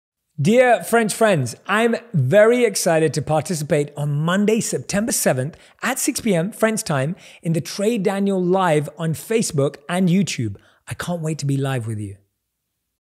Un message de @jayshetty pour les français ! RDV le 7 septembre à 18h !
Un message de @jayshetty pour les français !
L'auteur, podcasteur et ancien moine aux 30 millions de followers sera en Live sur nos pages Facebook et Youtube
le 7 septembre à 18h, pour la sortie de son livre événement « Penser comme un moine »
Retrouvez lors de ce live :
Franck Leboeuf
David Laroche
Jenna Blossoms
David Galley